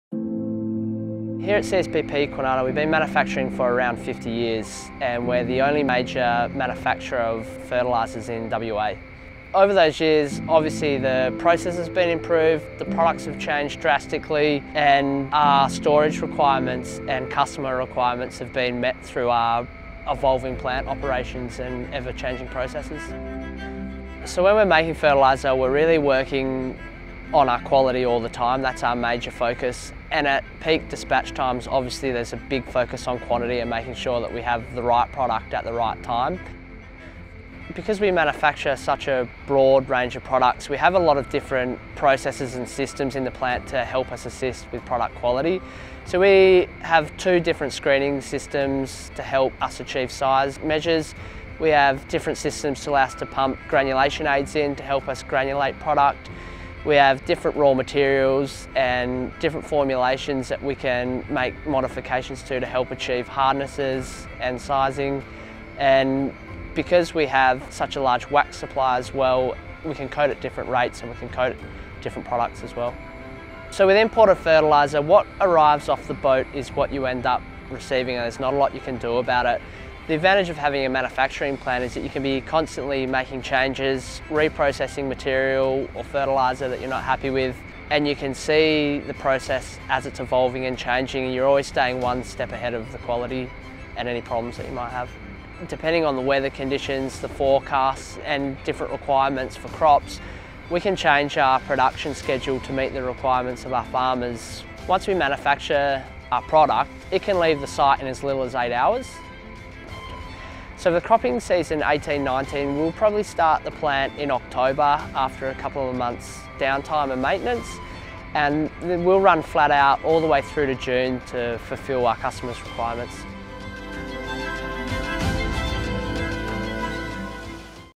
Behind the fertiliser granule
Ever wondered how your cropping fertiliser is manufactured? Come behind the scenes at our Kwinana manufacturing plant